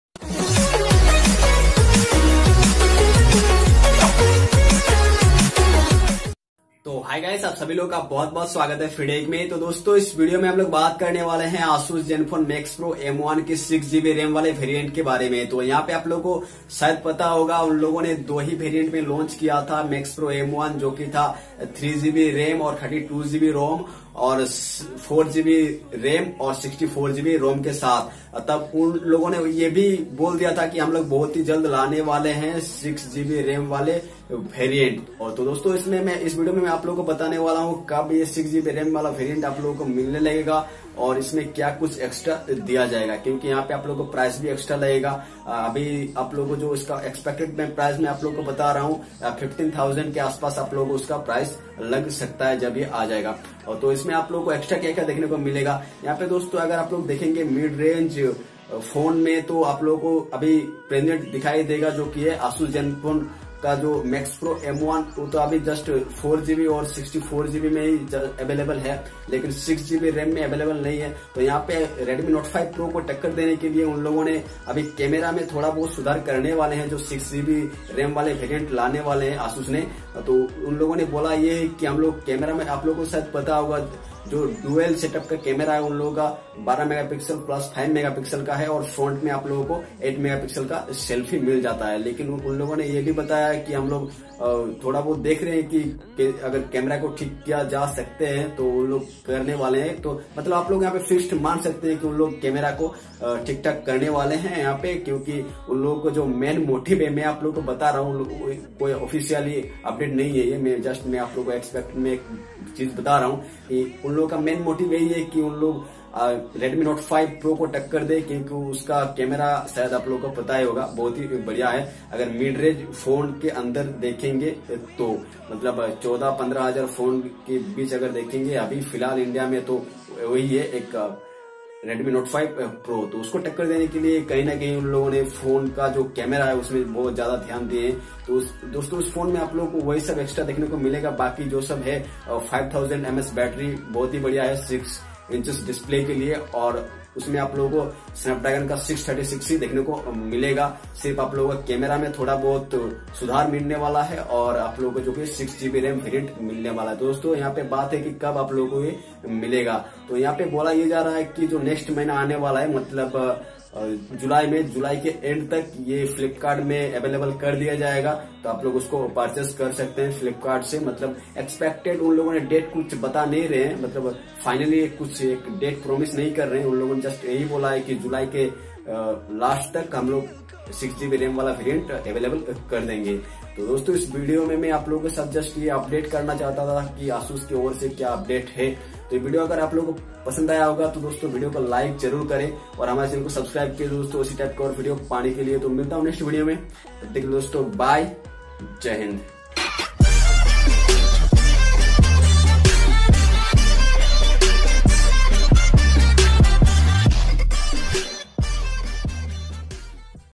Asus Zenfone Max Pro M1 6GB RAM Variants Available Price & Date Details
WELCOME TO FREE TECH

Hey guys in this video i talking about Asus Zenfone Max Pro M1 6gb Ram Variants Available date and price details.

It's comes with better camera and available date is expected end of July 2018